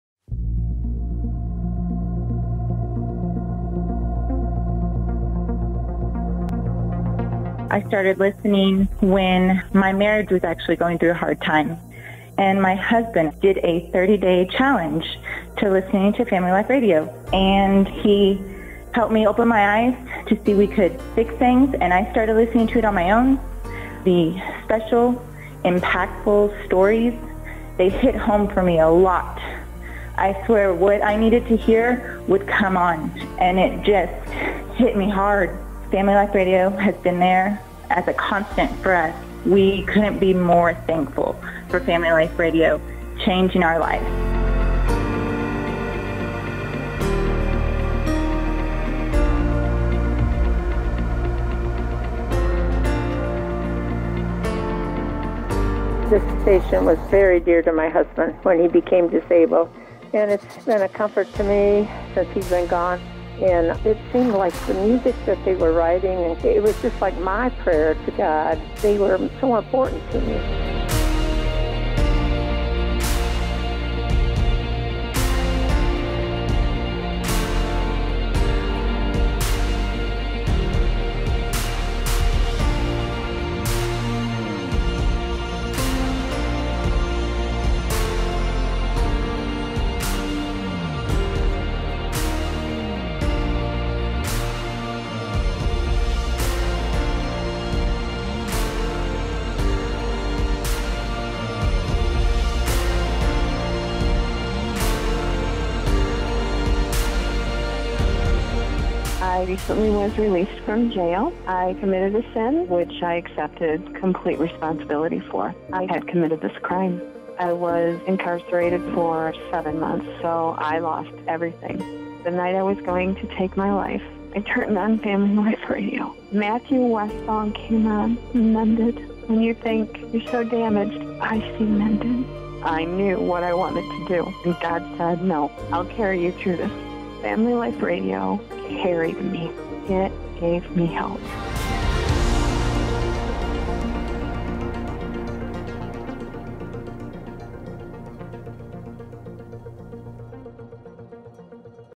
Family Life Radio // Reach and Impact
Look at all that God has accomplished through Family Life Radio in 2019.